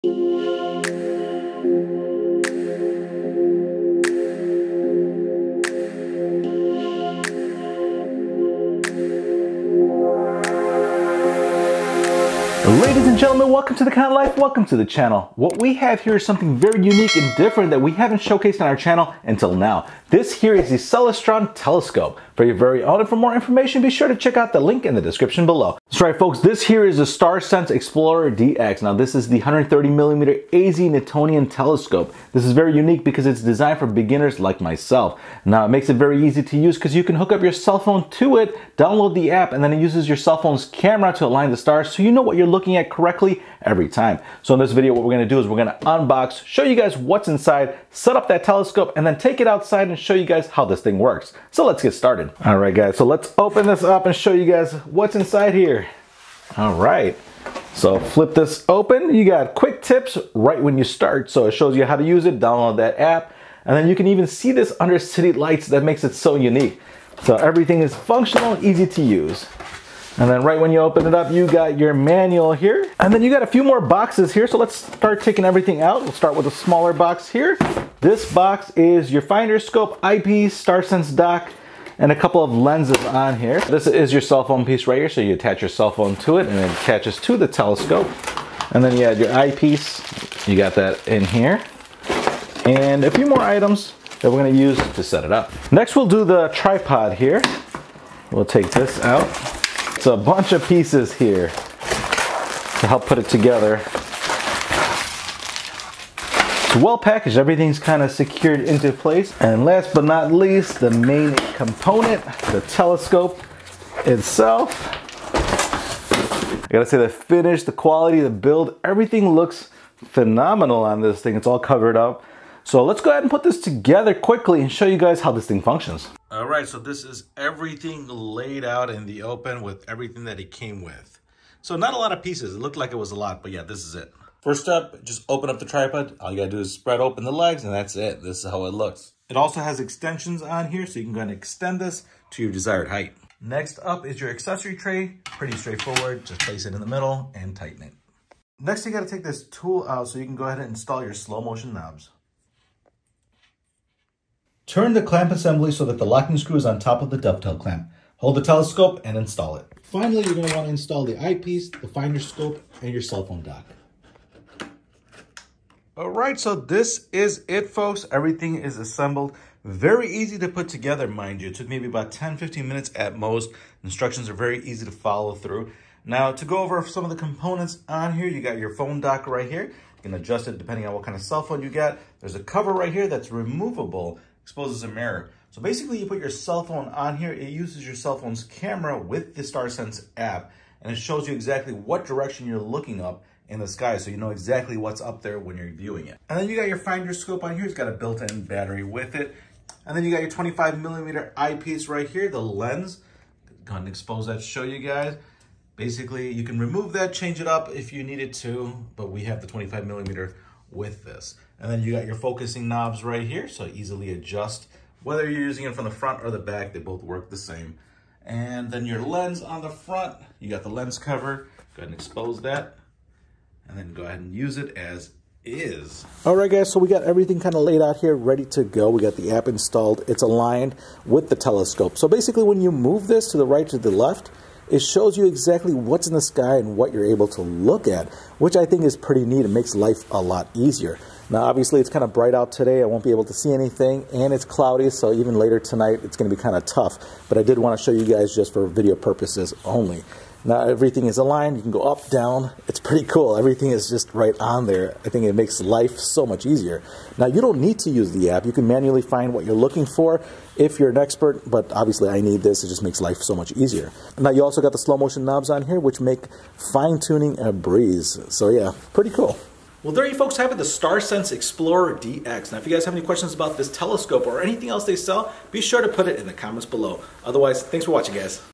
Celestron StarSense Explorer DX 130mm AZ Newtonian Telescope (Unboxing, assembly, App and use)
today we look at one of the most user friendly telescopes out there! this is the Starsense explorer DX by Celestron.  A budget friendly telescope that works really well! combine it with the Celestron App and it makes finding stars, planets a breeze!